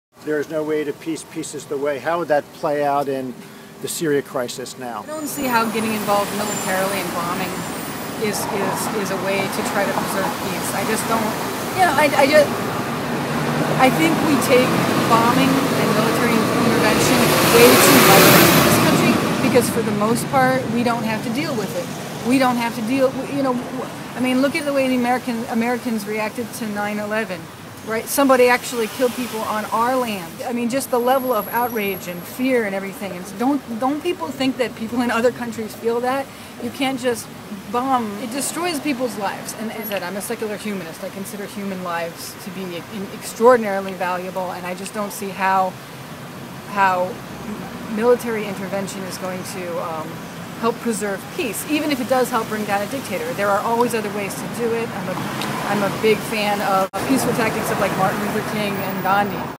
Peace is the way: don't bomb Syria
On her car's bumper this sticker: "There is no way to peace. Peace is the way." While the President was considering bombing Syria, Martha Knox disagrees.